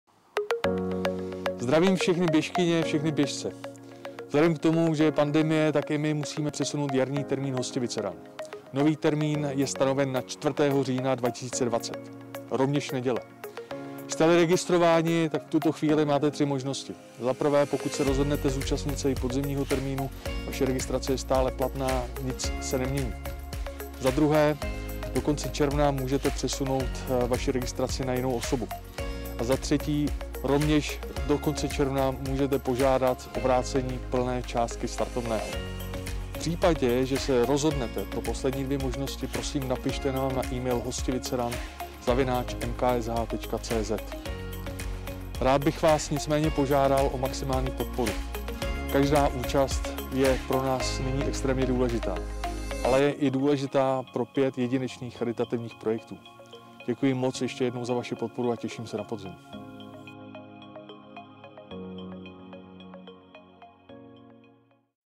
Hostivice RUN 2020 - nový termín 4. října
Vzhledem k situaci týkající se pandemie COVID-19 jsme nuceni změnit termín 7. ročníku Hostivice RUN 2020.